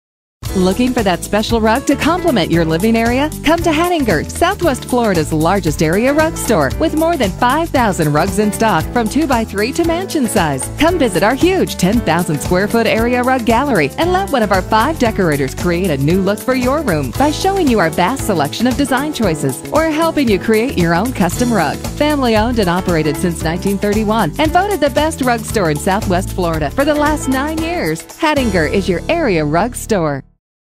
Hadinger for Area Rugs in Southwest Florida (Naples)
Largest area rug dealer from 2'x3' to mansion size. Come in and browser our gallery with over 5,000 rugs in stock. We can even help you create your own custom area rug.